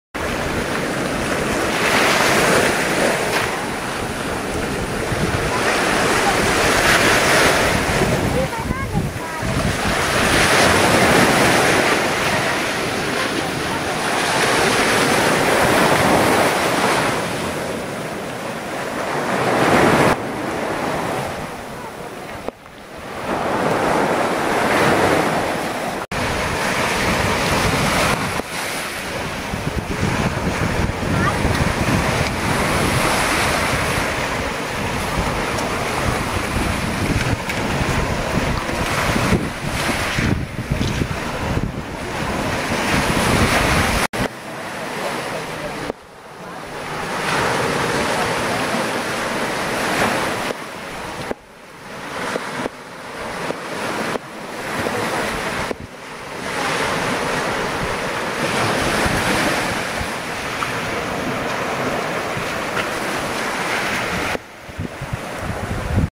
Il mare presso l'eremo Adonai a Brucoli
La costa orientale della Sicilia nei pressi di Brucoli. Il mare color turchese e il verde intenso della campagna, fanno da scenario per l'eremo e il santuario della Madonna Adonai , il primo tempio mariano del mondo occidentale risalente al Terzo secolo.